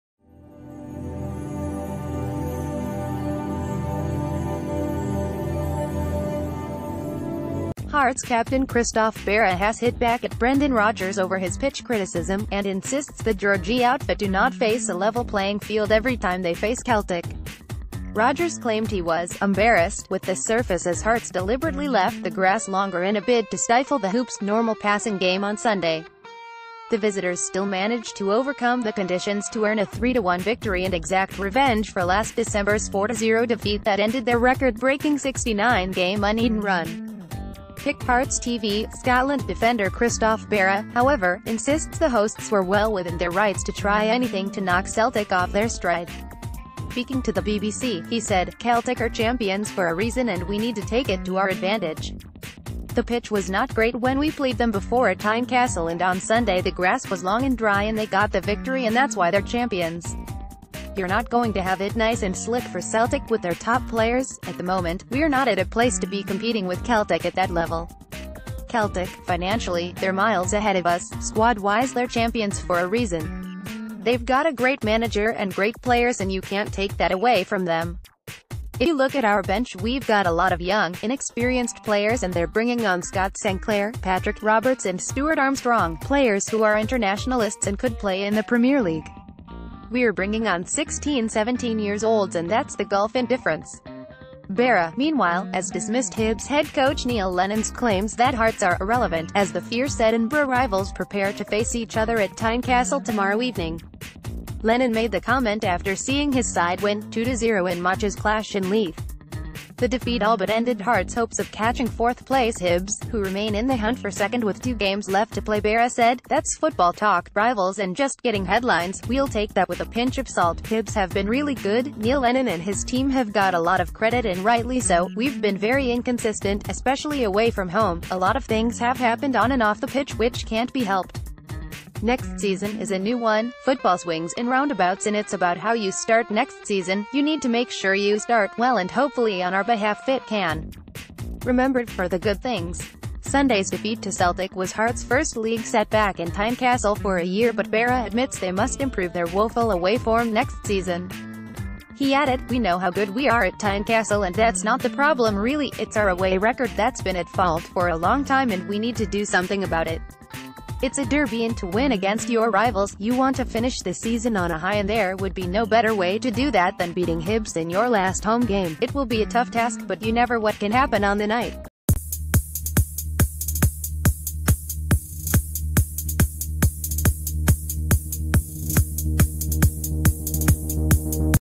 Breaking News | Hearts captain Christophe Berra responds to Celtic manager’s remarks
Hearts captain Christophe Berra has hit back at Brendan Rodgers over his pitch criticism, and insists the Gorgie outfit do not face a level playing field every time they face Celtic. Rodgers claimed he was ‘embarrassed’ with the surface as Hearts deliberately left the grass longer in a bid to stifle the Hoops’ normal passing game on Sunday. The visitors still managed to overcome the conditions to earn a 3-1 victory and exact revenge for last December’s 4-0 defeat that ended their record-breaking...